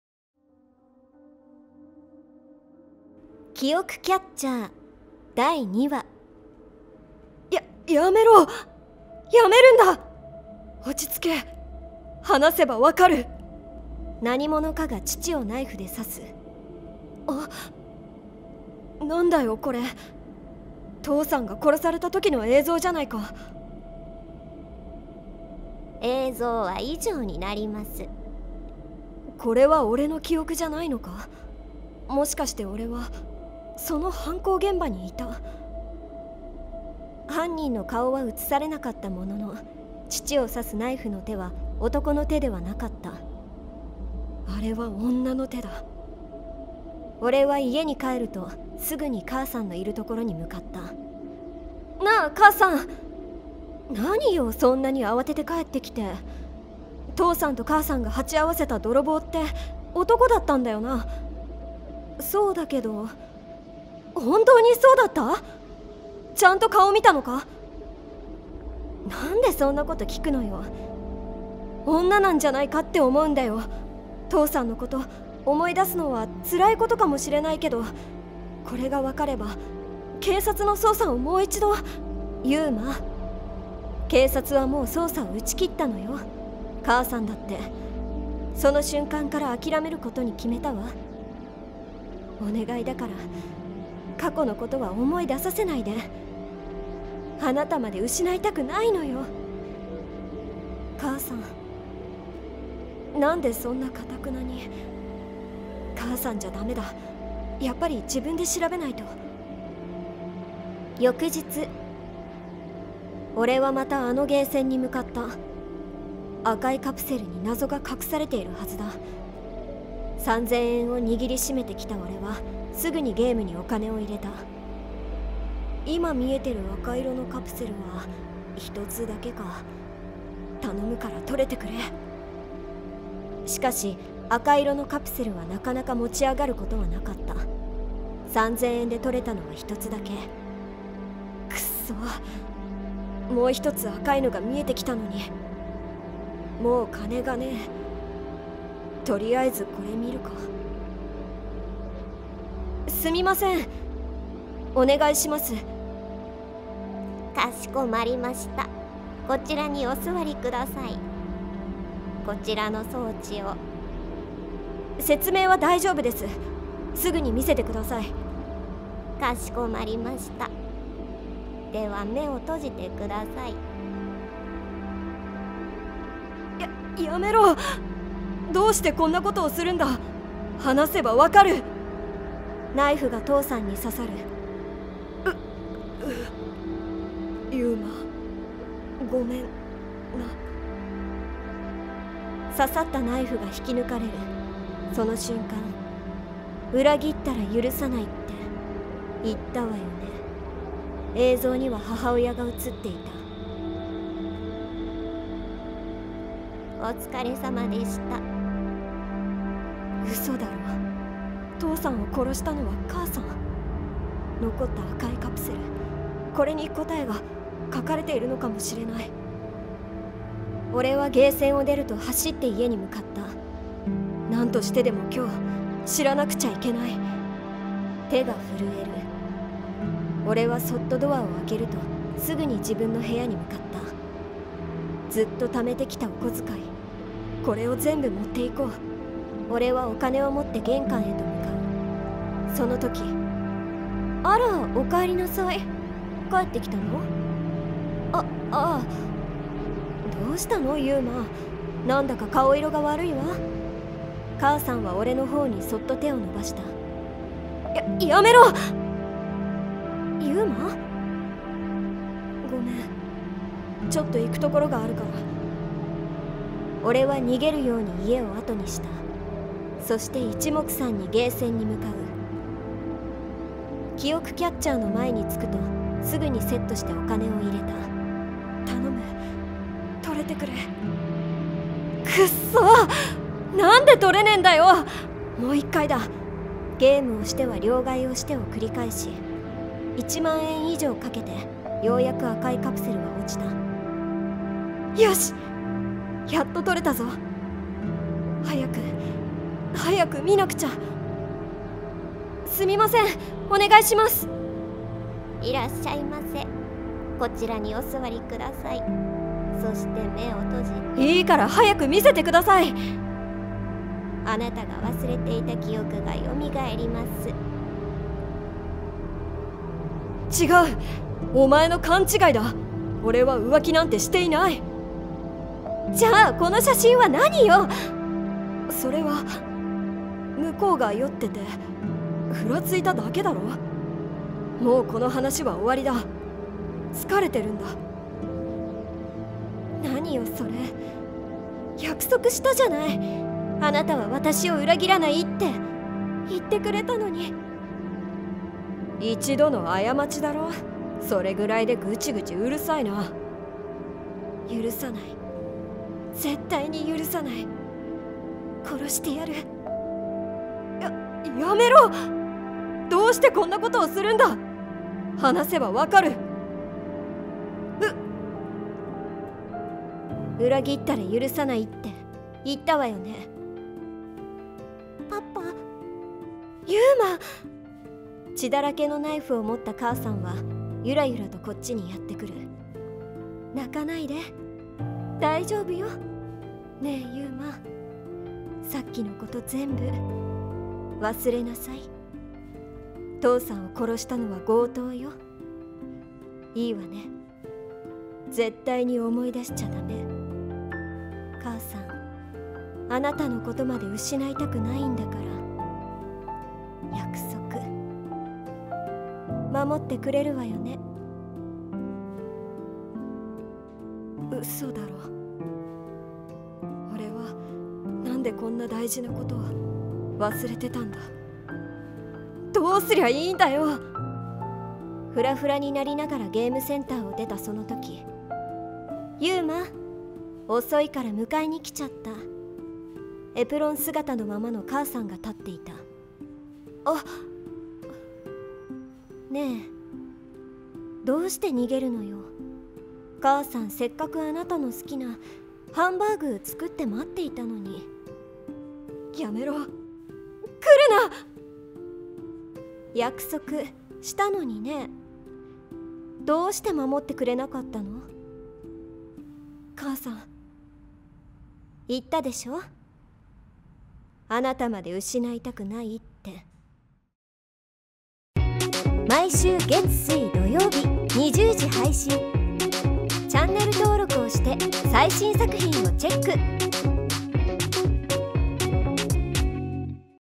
過去の記憶を思い出せるUFOキャッチャーがあると聞いて…【記憶キャッチャー】後編
記憶キャッチャーで幼い頃の記憶が見せてくれたのは、
残酷な真実だった…